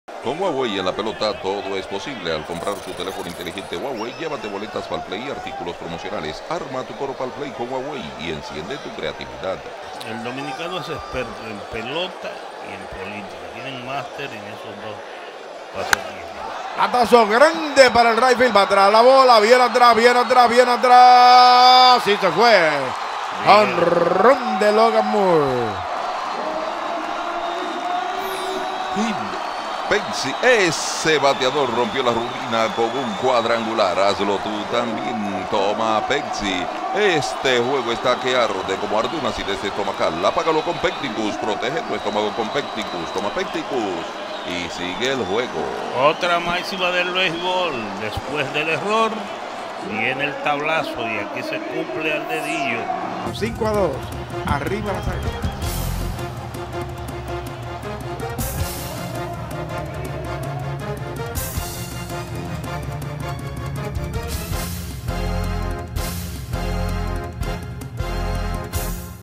Jonrón de Logan Moore @LoMo_10 vs. Águilas Cibaeñas. 07-12-15. ‪#‎LiceyTv‬ ‪#‎Licey‬
Jonrón de Logan Moore en la 3ra entrada del partido vs. Águilas Cibaeñas. 07-12-15. ‪#‎LiceyTv‬ ‪#‎Licey‬